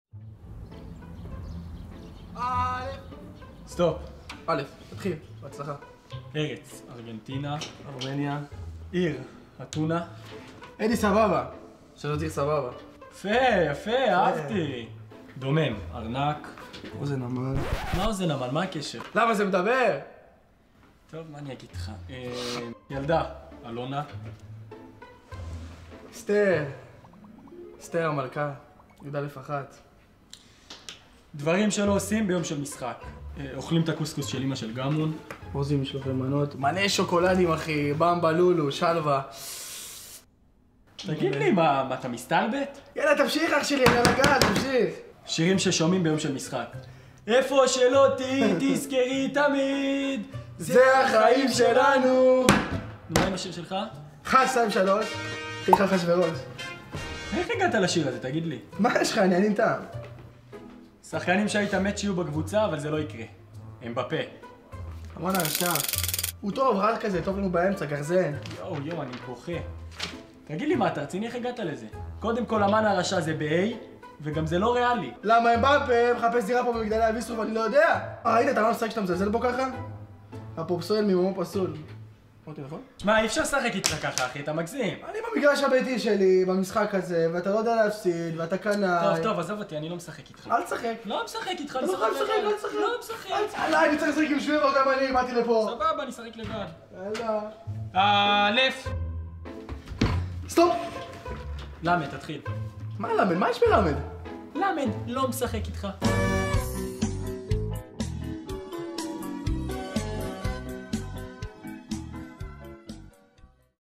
הפועל באר שבע עושים שקופים - פורים 2021
מה קרה כשרותם חטואל ותומר יוספי שיחקו ארץ עיר?